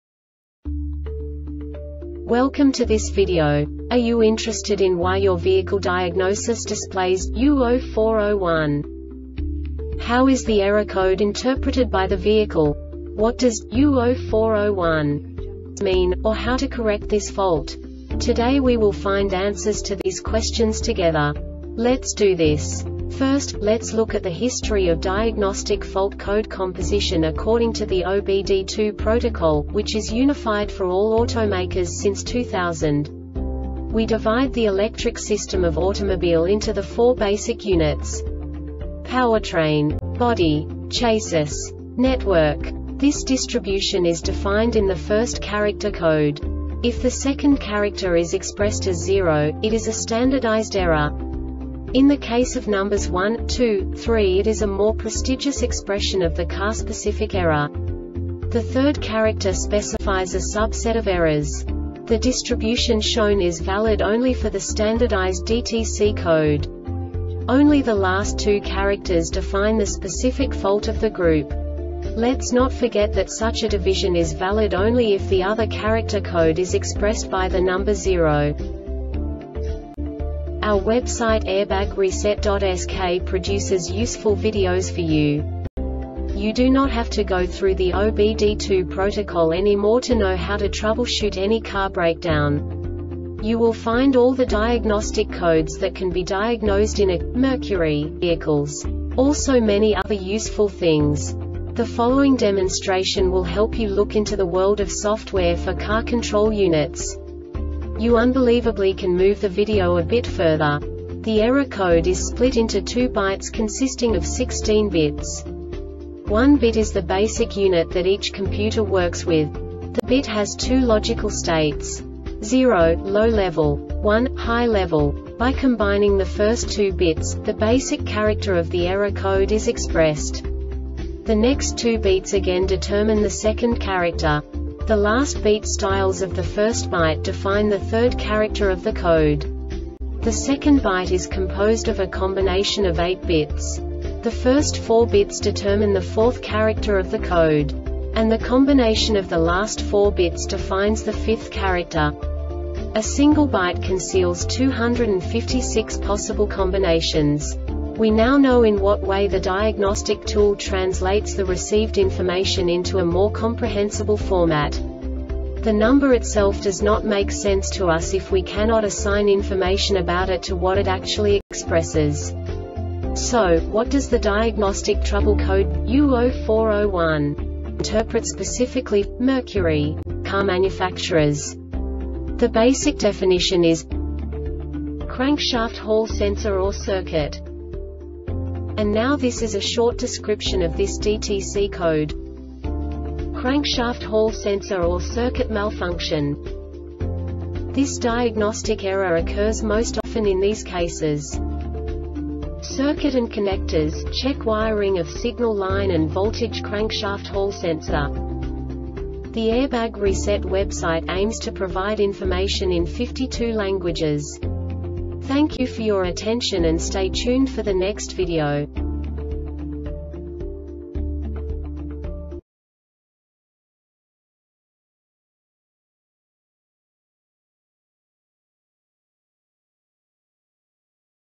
DTC Mercury U0401 Short Explanation
Contents:
0:21 Basic DTC analysis according to OBD2 protocol standard.
1:48 Insight into programming
2:58 A diagnostically understandable description of the Diagnostic Trouble Code
3:05 Basic error definition

Make:
Mercury
Code:
U0401
Definition:
Crankshaft Hall Sensor or Circuit
Description:
Crankshaft Hall sensor or circuit malfunction
Cause:
Circuit and connectors: - Check wiring of signal line and voltageCrankshaft Hall sensor